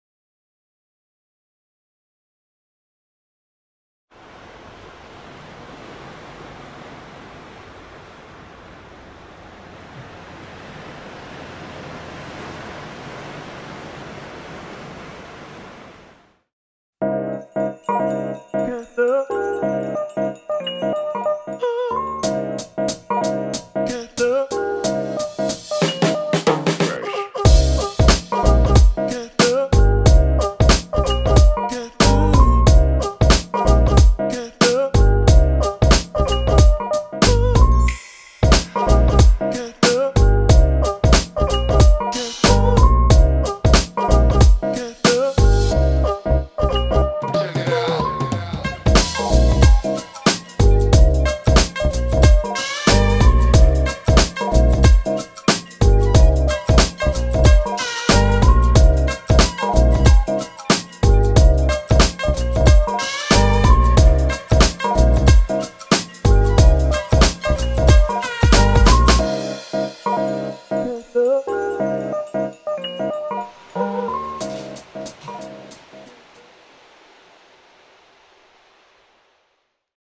ニューノーマルの暮らしをデザインする ATELIER OPA熱海サテライトオフィス Designing New Normal, Satellite office in Atami
株式会社ATELIER OPAは東京のデザイン事務所です。静岡県熱海市の海沿いにサテライトオフィスを設けました。YODOKO＋のHOME OFFICEなど自社デザイン製品を用いたワーケーションをご紹介します。
Music from Uppbeat (free for Creators!)